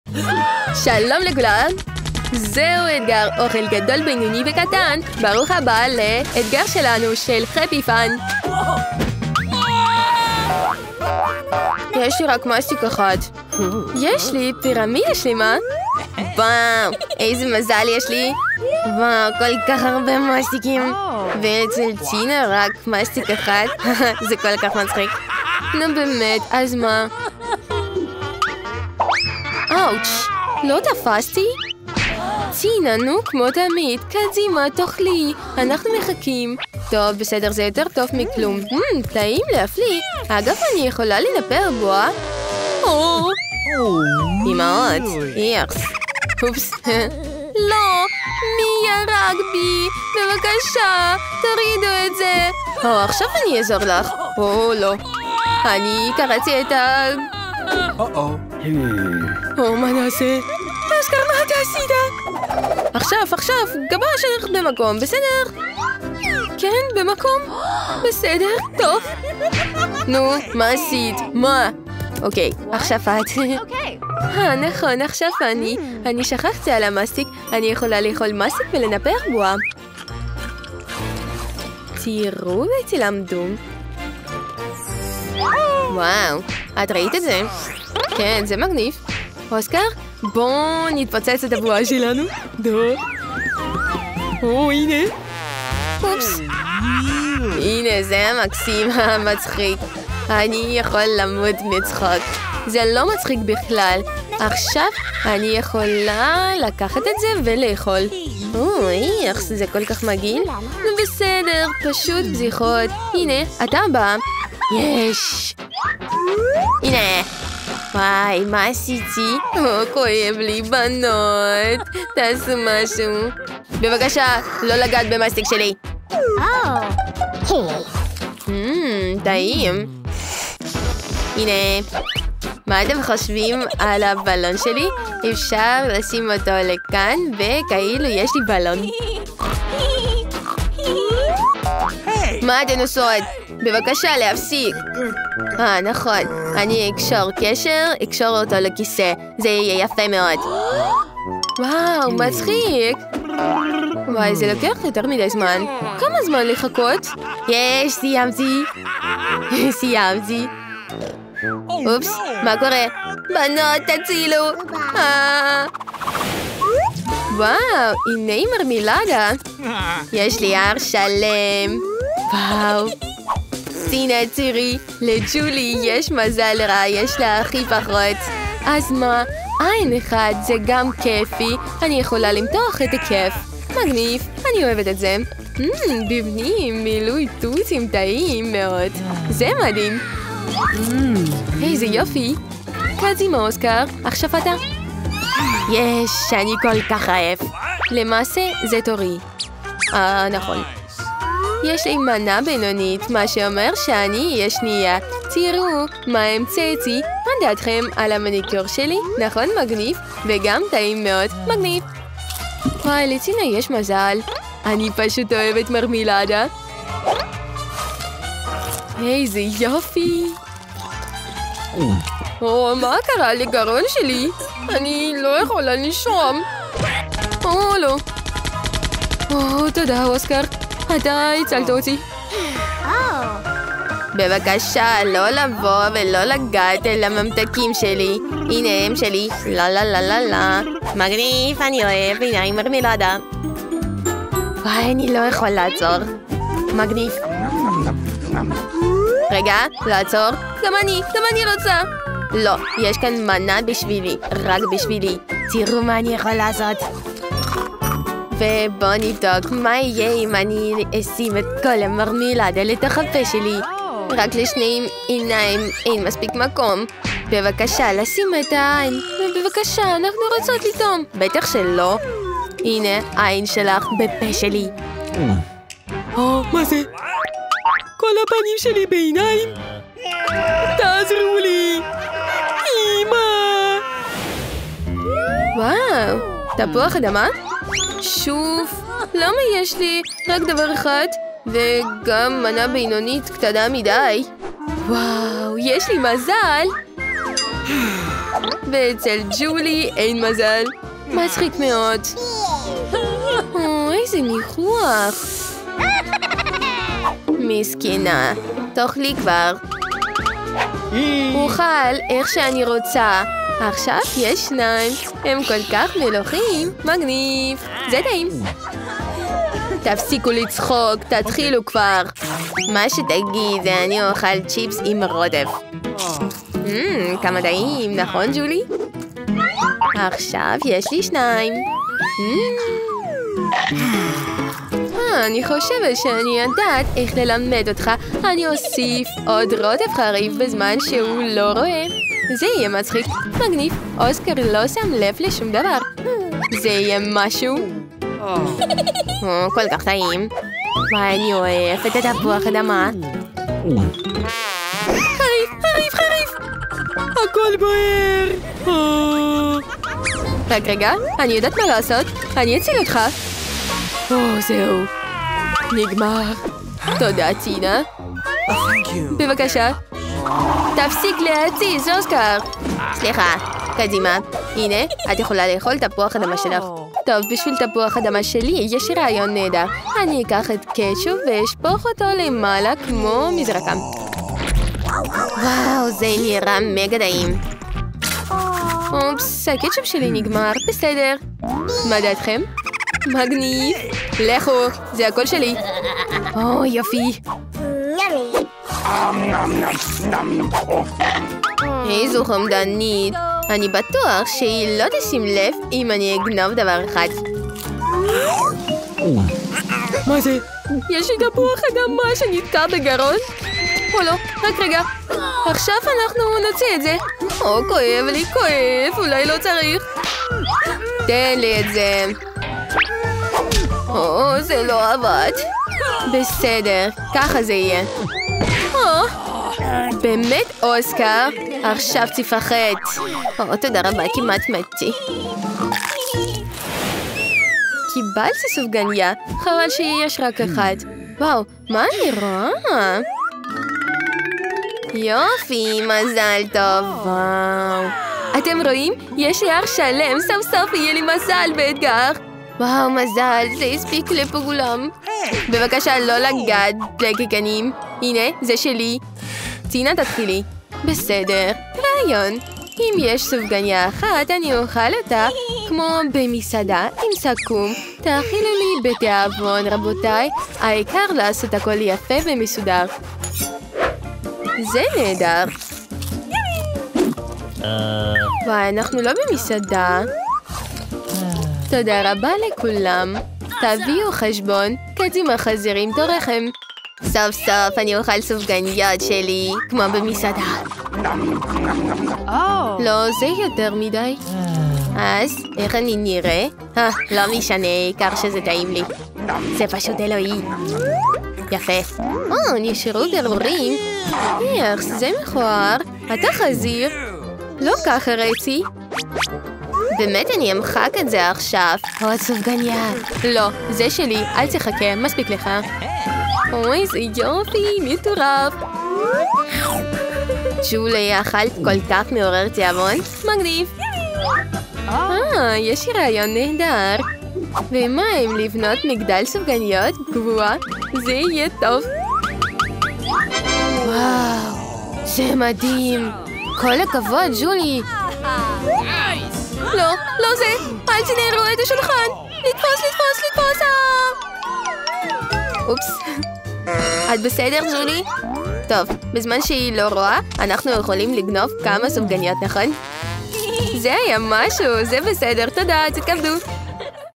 אתגר 100 שכבות של מזון Happy Fun
שפע של אוכל תמיד נהדר! יהיה הרבה מזה באתגר המעניין שלנו!

אל תשכח לשתף את הסרטון הזה עם חבריך ולהירשם לערוץ Happy Fun ביוטיוב כדי לראות עוד סרטונים מדהימים כמו זה!